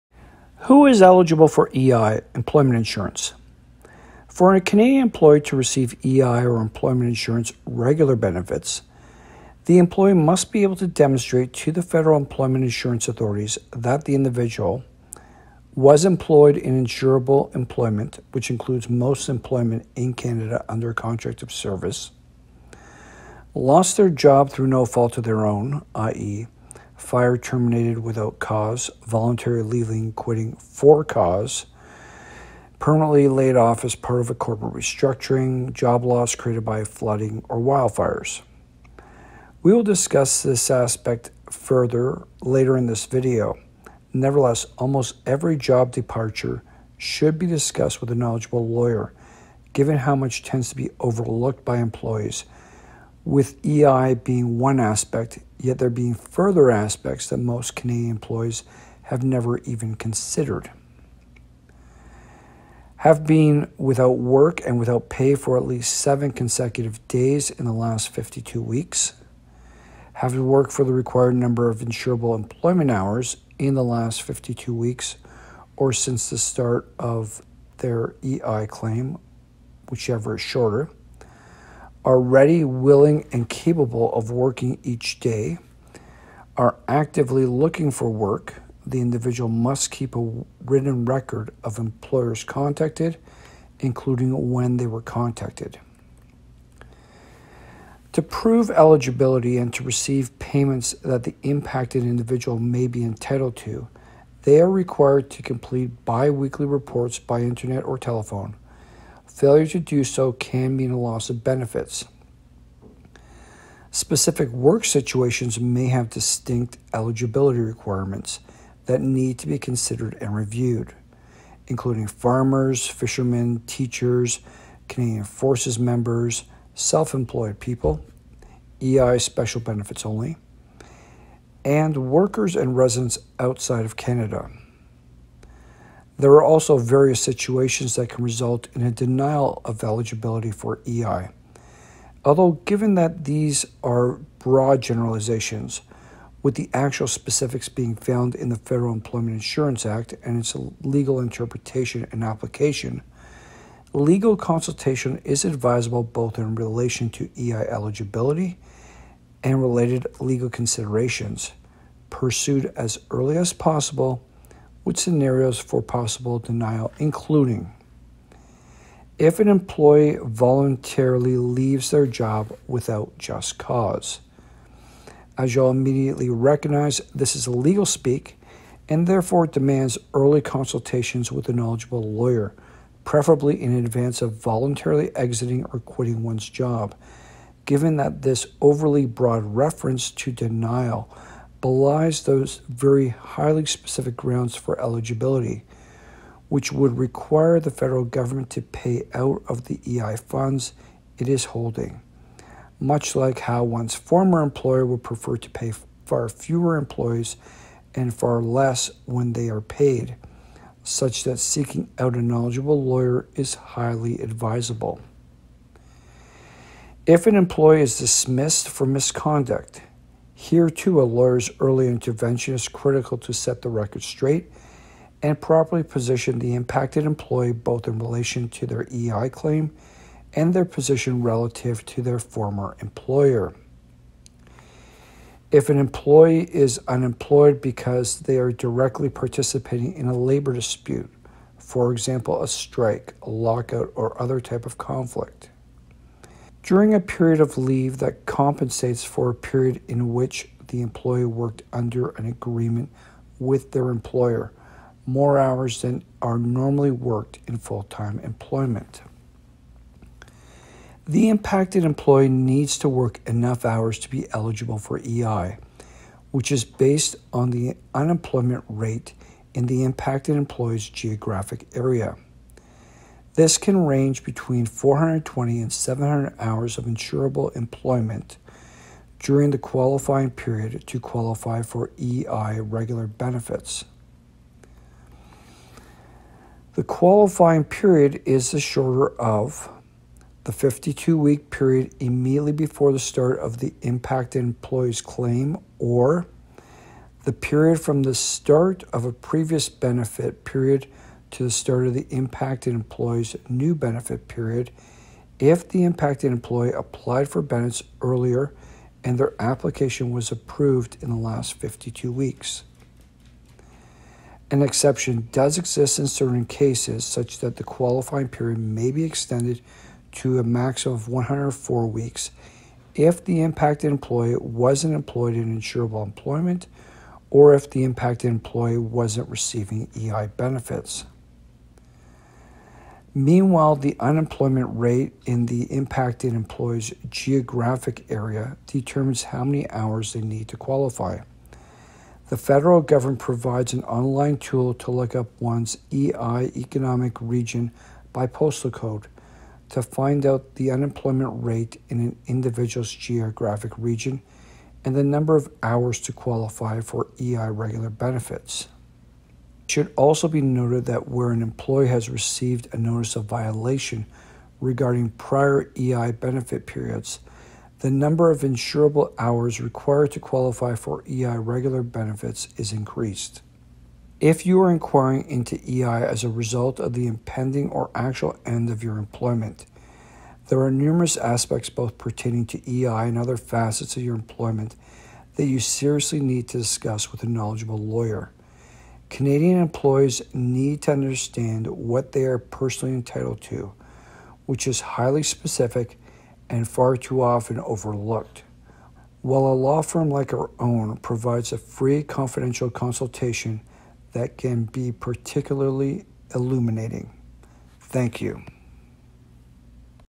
Who is Eligible for EI - Employment Insurance
Are you entitled to employment insurance? What are the criteria to receive EI regular benefits? What do employees who are out of work overlook when applying for EI?
For a Canadian employee to receive EI or Employment Insurance Regular benefits, the employee must be able to demonstrate to the federal employment insurance authorities that the individual:
- was employed in insurable employment, which includes most employment in Canada under a contract of service
- lost their job through no fault of their own (i.e., fired/terminated without cause, voluntarily leaving/quitting for cause, permanently laid off as part of a corporate restructuring, job loss created by flooding or wildfires)
- have been without work and without pay for at least 7 consecutive days in the last 52 weeks
- have worked for the required number of insurable employment hours in the last 52 weeks or since the start of their last EI claim, whichever is shorter
- are ready, willing and capable of working each day
- are actively looking for work (the individual must keep a written record of employers contacted, including when they were contacted)
To prove eligibility and to receive payments that the impacted individual may be entitled to, they are required to complete bi-weekly reports by internet or telephone. Failure to do so can mean a loss of benefits.
Specific work situations may have distinct eligibility requirements that need to be considered and reviewed, including farmers, fishermen, teachers, Canadian Forces members, self-employed people (EI special benefits only), and workers and residents outside of Canada.
There are also various situations that can result in a denial of eligibility for EI, although given that these are broad generalizations, with the actual specifics being found in the federal Employment Insurance Act and its legal interpretation and application, legal consultation is advisable both in relation to EI eligibility and related legal considerations, pursued as early as possible, with scenarios for possible denial including:
- if an employee voluntarily leaves their job without just cause
- if an employee is dismissed for misconduct
- if an employee is unemployed because they are directly participating in a labour dispute (for example, a strike, lockout or other type of conflict)
- during a period of leave that compensates for a period in which the employee worked under an agreement with their employer, more hours than are normally worked in full-time employment
The impacted employee needs to work enough hours to be eligible for EI, which is based on the unemployment rate in the impacted employee’s geographic area. This can range between 420 and 700 hours of insurable employment during the qualifying period to qualify for EI regular benefits.
The qualifying period is the shorter of:
- the 52-week period immediately before the start date of the impacted employee’s claim, or
- the period from the start of a previous benefit period to the start of the impacted employee’s new benefit period, if the impacted employee applied for benefits earlier and their application was approved in the last 52 weeks
An exception does exist in certain cases, such that the qualifying period may be extended to a maximum of 104 weeks if the impacted employee wasn’t employed in insurable employment or if the impacted employee wasn’t receiving EI benefits.
Meanwhile, the unemployment rate in the impacted employee’s geographic area determines how many hours they need to qualify. The federal government provides an online tool to look up one’s EI Economic Region by Postal Code to find out the unemployment rate in an individual’s geographic region and the number of hours to qualify for EI regular benefits.
It should also be noted that where an employee has received a notice of violation regarding prior EI benefit periods, the number of insurable hours required to qualify for EI regular benefits is increased.